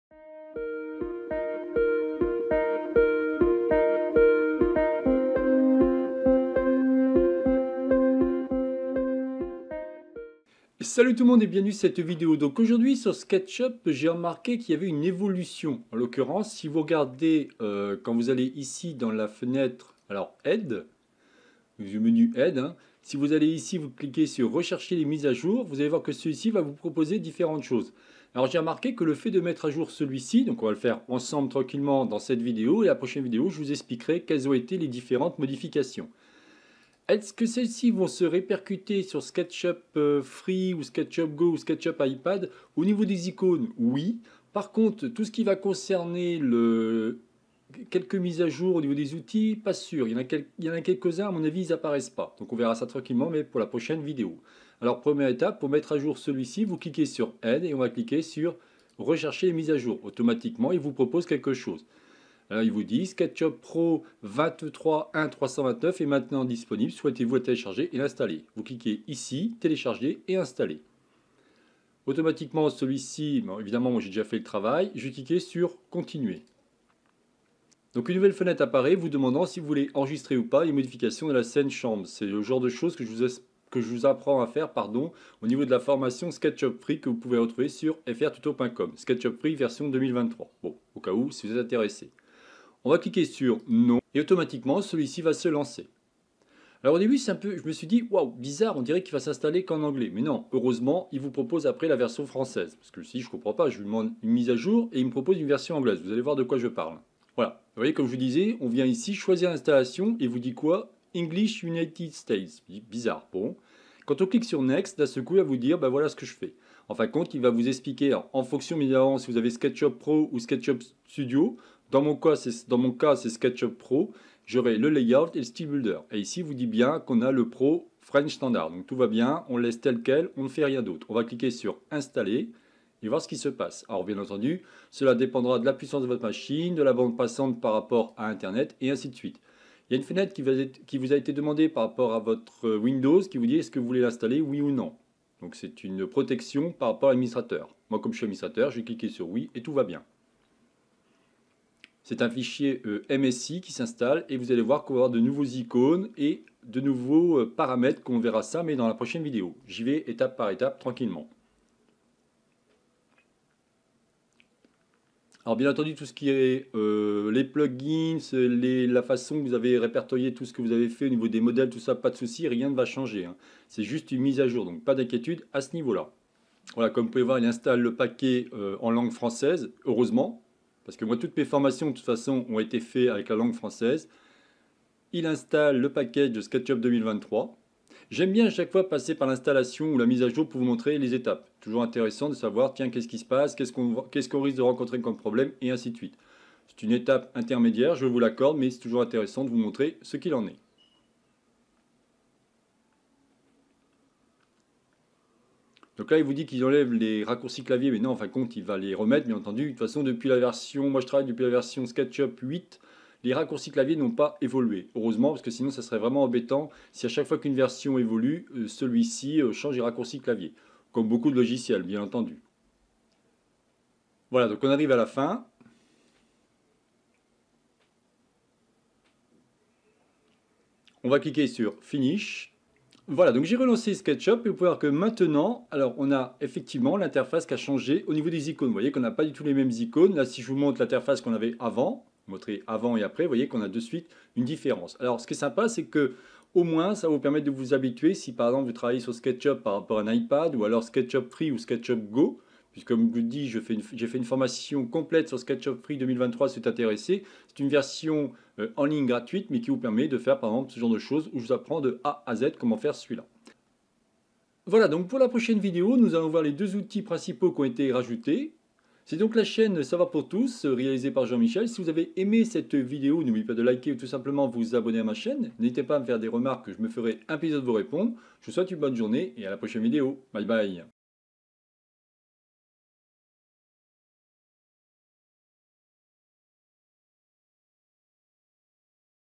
Sketchup 2023 - Mise à jour (nouveaux icônes)
Sketchup n'arrête pas d'évoluer, cette fois-ci Skechup a harmonisé la gestion et l'apparence des icônes que cela soit sur Sketchup Free, Go ou Pro afin que l'on ne soit plus dépaysé par la mise en place des icônes, nous allons donc commencer par la mise à niveau de Sketchup pour Septembre 2023.